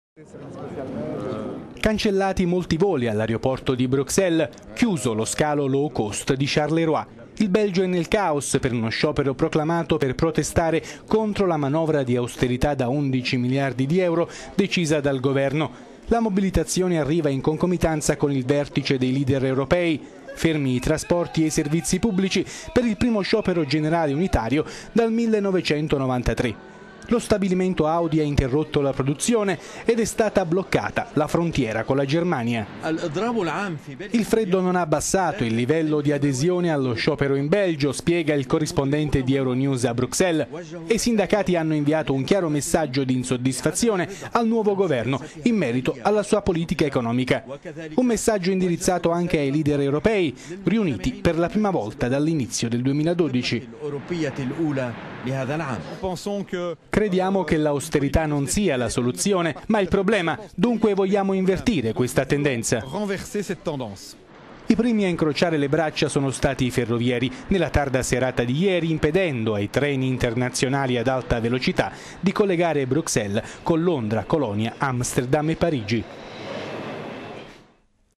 Sciopero in Belgio, bloccata frontiera con la Germania
Cancellati molti voli all'aeroporto di Bruxelles, mentre lo scalo dei voli low cost a Charleroi è chiuso.

Il Belgio è  nel caos per uno sciopero proclamato per protestare contro la manovra di austeriità da 11 miliardi di euro decisa dal governo. La mobilitazione

arriva in concomitanza con il vertice dei leader europei.

Fermi i trasporti e i sevizi pubblici per il primo sciopero generale unitario dal 1993.

Lo stabilimento Audi ha interrotto la produzione ed è stata bloccata la frontiera con la Germania.